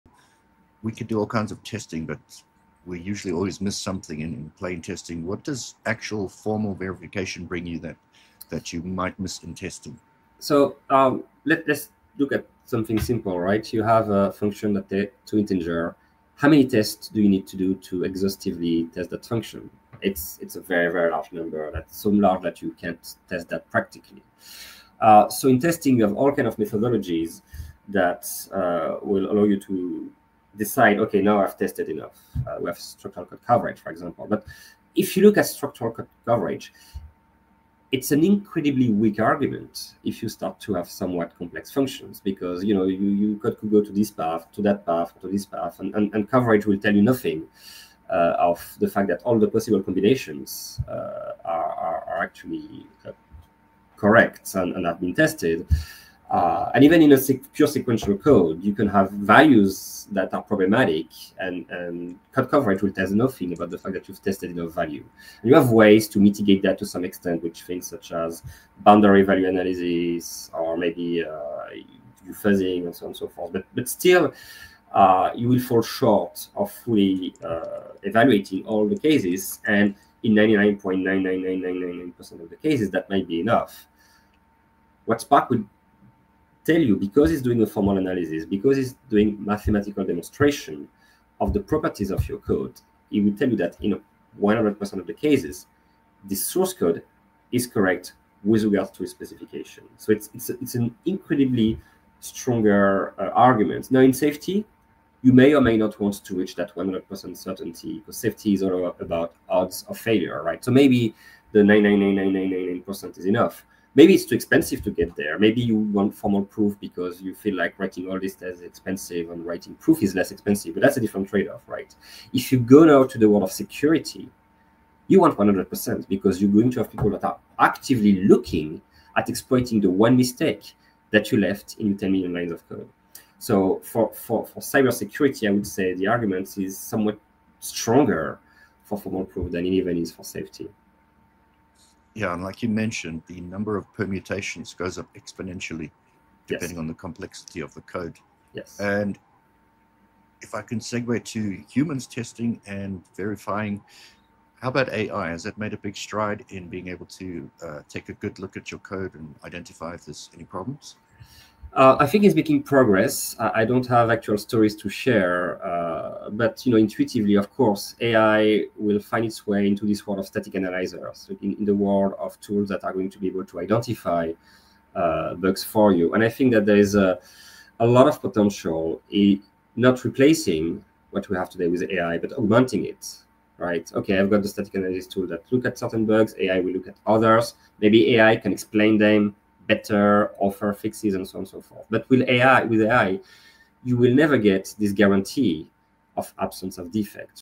Why Testing Can Never Be 100% - The Math Behind Software Verification
In this snippet from Elektor Engineering Insights #54, we explore why traditional software testing can never guarantee 100% reliability and how formal verification provides mathematical certainty that testing simply cannot achieve. Quentin Ochem from AdaCore explains the fundamental limitation of testing - even a simple function with two integers would require an astronomically large number of test cases to verify exhaustively - and how formal verification tools such as Spark use mathematical proofs to demonstrate software correctness across ALL possible execution paths with 100% certainty, which is especially critical for security applications where attackers only need to find one exploit in millions of lines of code.